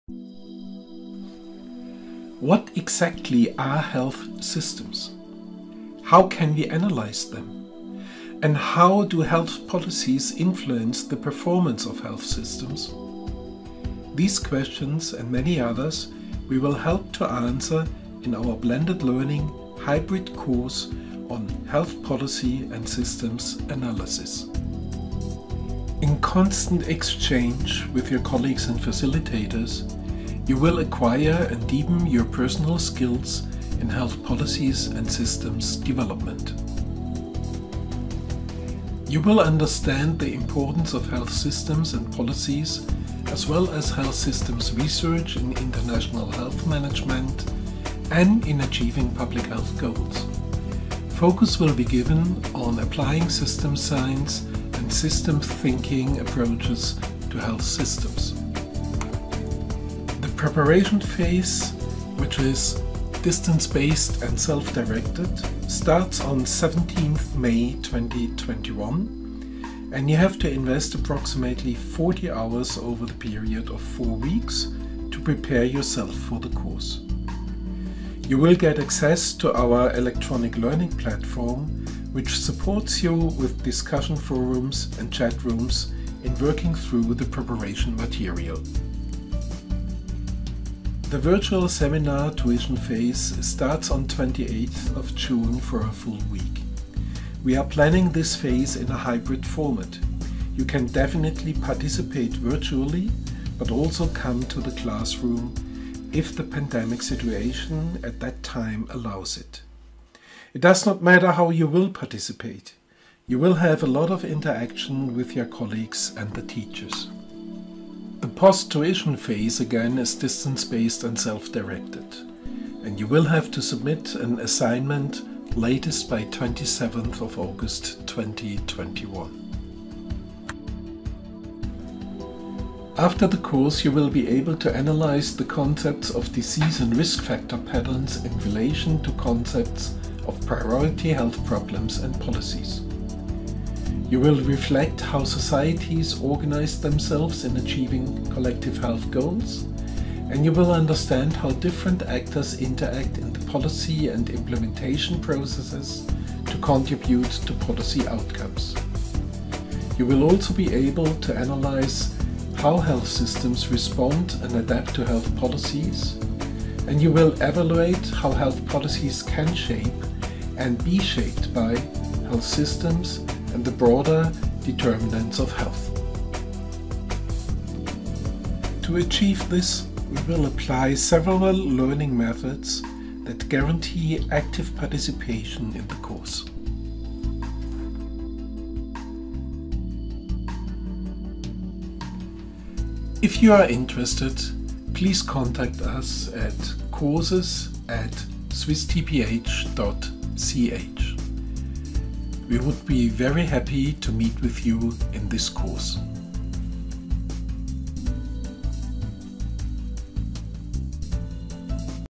Health Policy and System Analysis
Short course and part of the MBA in International Health Management (MBA-IHM) of the Swiss Tropical and Public Health Institute (Swiss TPH).
The course analyses the relationships between global health strategies and national and local health policies and systems development. It focuses then on health systems frameworks, the role and behaviour of institutions and stakeholders, the major innovations in financing and delivering health services, and on how health systems are responding to global changes.